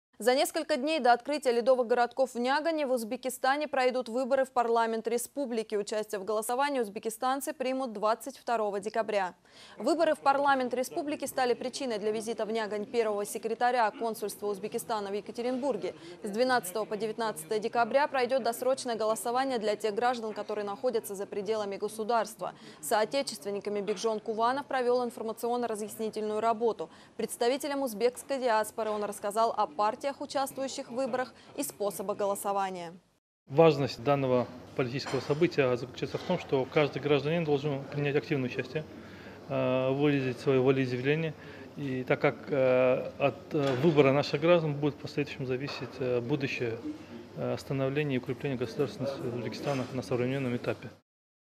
Парламентские выборы Узбекистана 2019
Первый секретарь Генконсульства Узбекистана провел встречу с соотечественниками в г. Нягань. Основной темой было информирование о Парламентских выборах Узбекистана 2019 г.  и проведение досрочного голосования.